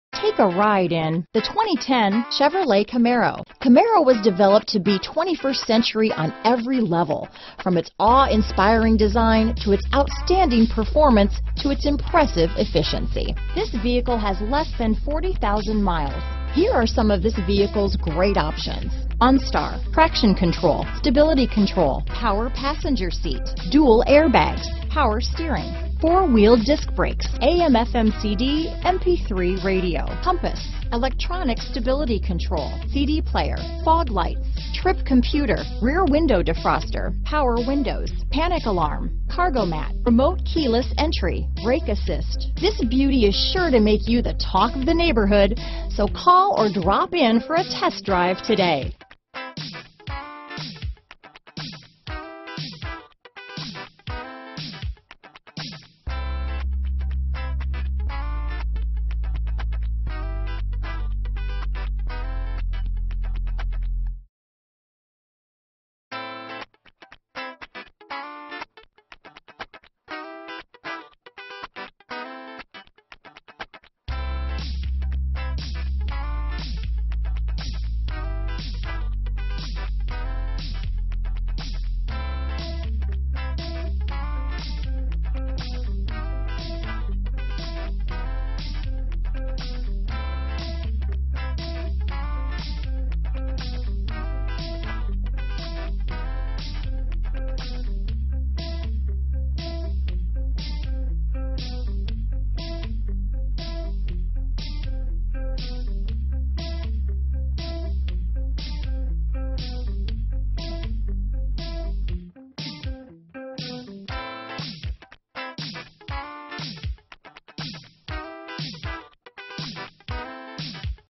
2010 Chevrolet Camaro Schaumburg, Arlington Heights, Hoffman Estates, Des Plaines, Palatine, IL 1583
Used 2010 Chevrolet Camaro available in Schaumburg, Illinois at Schaumburg Honda. Servicing the Arlington Heights, Hoffman Estates, Des Plaines, Palatine, IL area.

Schaumburg Honda
750 E Golf Rd

Recent Arrival! 2010 Chevrolet RWD Camaro 1LT 6-Speed Odometer is 64265 miles below market average!